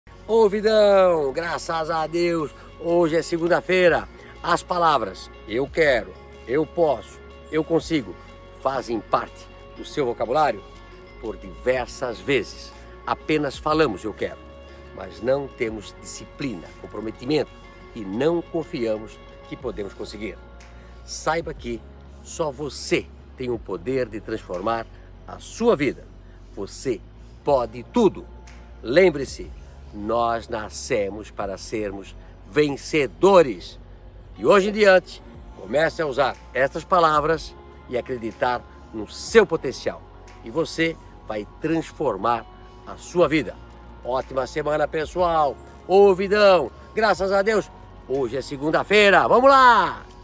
Ô VIDÃO | EU QUERO, EU POSSO, EU CONSIGO | LUCIANO HANG
Ô Vidão, graças a Deus hoje é segunda-feira! Como anda sua autoestima? E a confiança em si mesmo? Com disciplina e determinação, podemos muito. Mas o primeiro passo, acima de tudo, é acreditar em nosso potencial. Fale mais sobre sua capacidade, as palavras têm poder. Mas não se esqueça, sem ação, nossa intenção é inútil. Comece hoje mesmo, coloque todas suas forças naquilo que acredita, você é o único responsável por transformar a sua vida. Ótima semana, pessoal! Forte abraço.